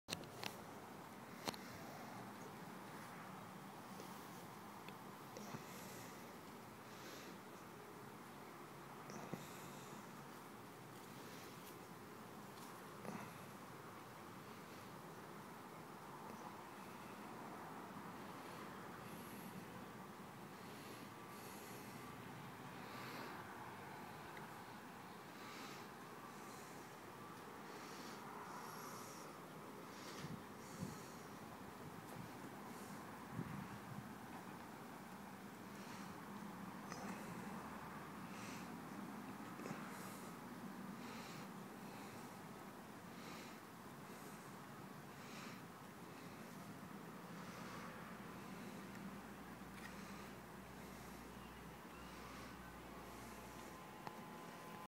Layer 3 clear coat lacquer
Third and final layer of Behlen stringed instrument lacquer, effectively 9 layers: 3 layers per day times 3 days, this one got a bug in it, flew into paint stream and BAM into a corner. Stopped, removed it, layered a bit heavy on the spot 2 times extra. Figures on my final clear.